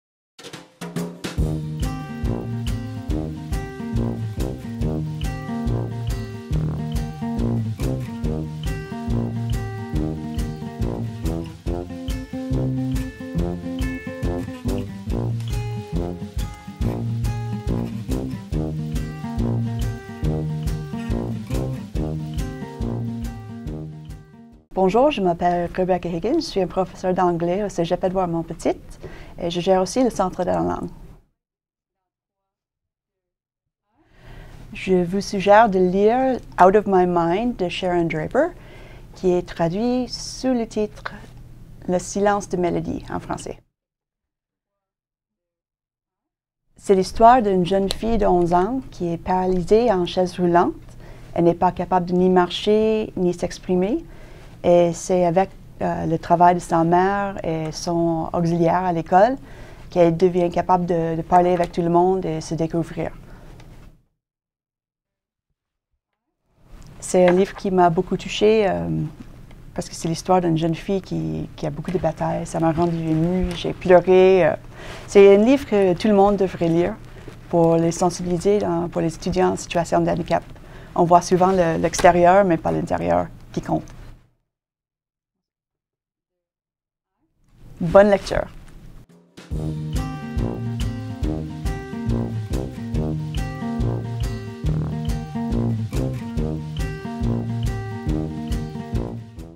Coup de coeur de lecture
Que lisent les gens du cégep Édouard-Montpetit? Quels livres vous suggèrent-ils de lire et pourquoi? Découvrez la suggestion de lecture de la professeure au Département de langues du cégep Édouard-Montpetit, Rebecca Peters.
La réalisation de cette vidéo a été coordonnée par le Service de la recherche et du développement en collaboration avec le Département de littérature et français et la Direction des communications du cégep Édouard-Montpetit.